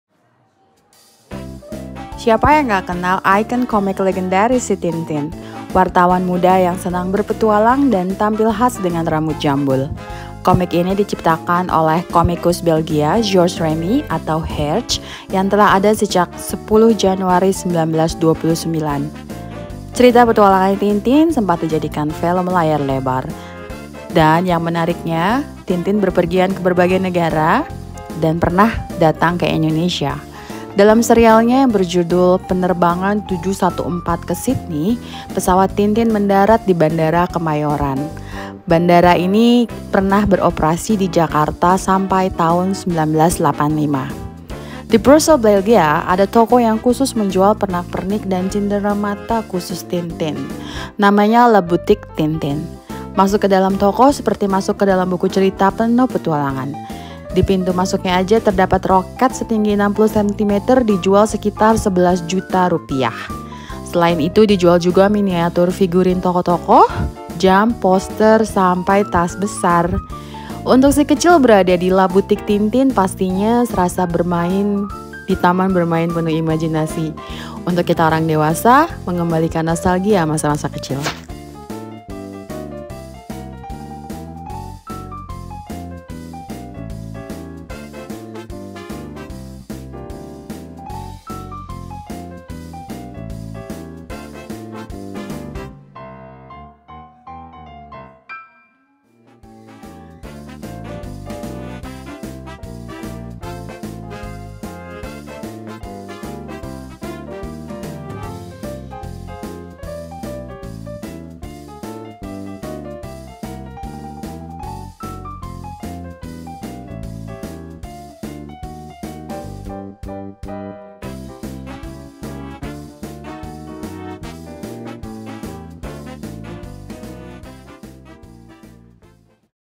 Pengalaman Fans ke Butik Tintin di Belgia, Isinya Bikin Mata Berkaca-kaca!
Masuk ke dalam toko ini seperti masuk ke dalam buku cerita penuh petualangan, di pintu masuknya saja terdapat roket setinggi 60 centimeter seperti dalam cerita Tintin yang berjudul ‘Petualangan ke bulan’. Roket ini dijual seharga 650 Euro atau sekitar Rp10 jutaan.

Selain itu dijual juga miniatur figurin tokoh-tokohnya, jam, poster, tas sampai jam tangan. Tas besar bergambar siluet Tintin dijual seharga 5 Euro dan jam tangan dijual seharga mulai dari 69 Euro.

Official Channel of INDOZONE

LINE Official: @INDOZONE (use ‘@’)